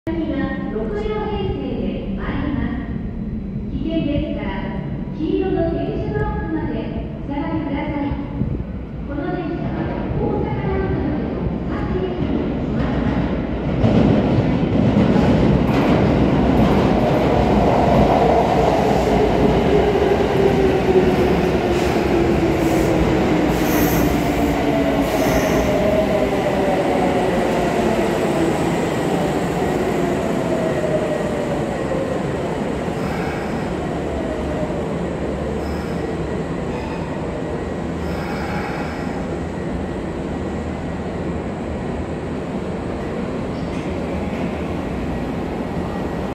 急行 大阪難波行き到着!! 近鉄8400系+近鉄1233系 6両編成
大阪上本町駅にて。
大阪難波行き急行が到着します。